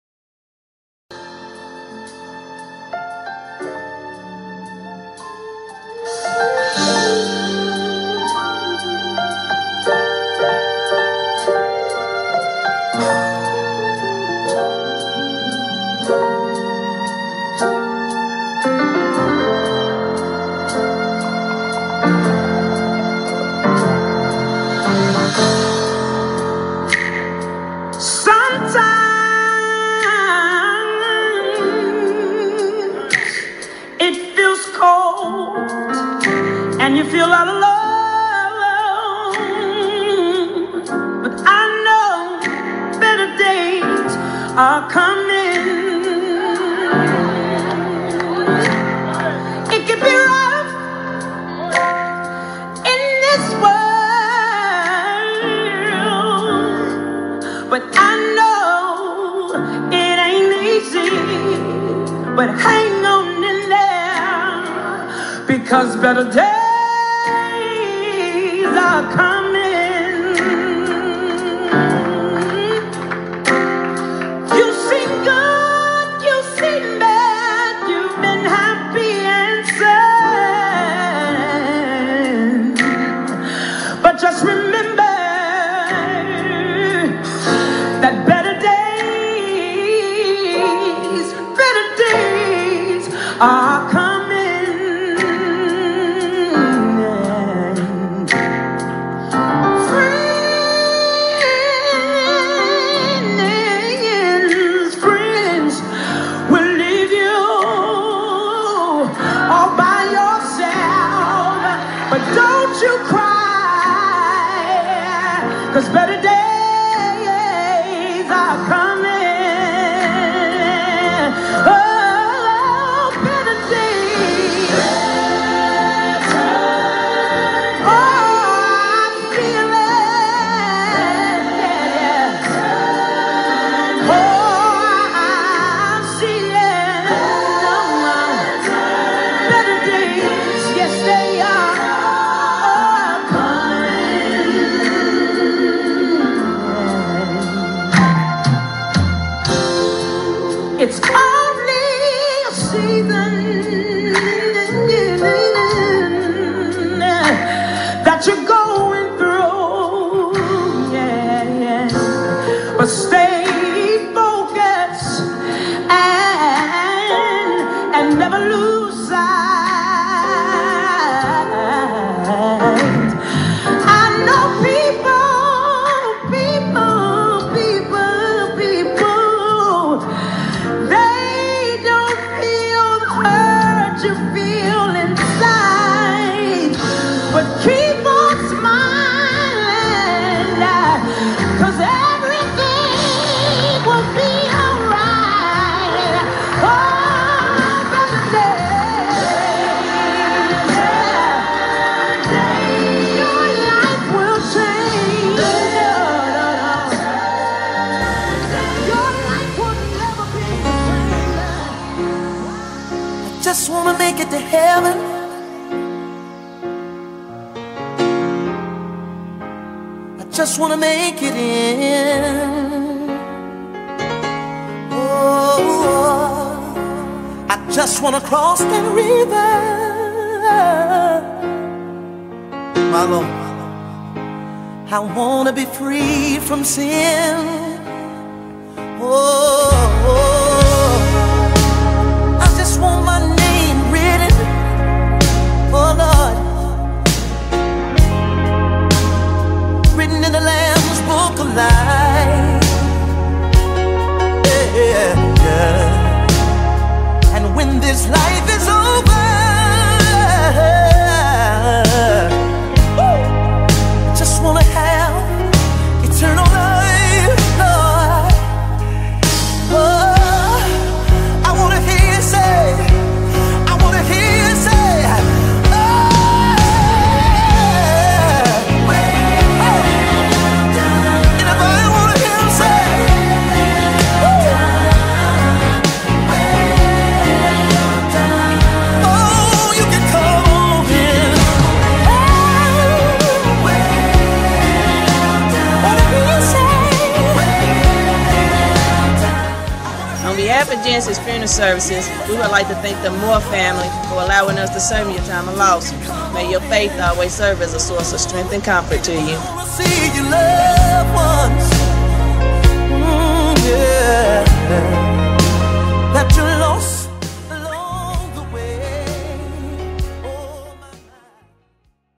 Home Going Celebration For Our Beloved Janice Fay Moore
The family of Janice Fay Moore would like to express sincere appreciation for all those who were able to attend the home going. They would also like to show gratitude for those who sent their thoughts and prayers but were unable to be present by providing a visual commemorative for view now and for years to come. Please express your continued support of the family by liking their video. Produced BY Don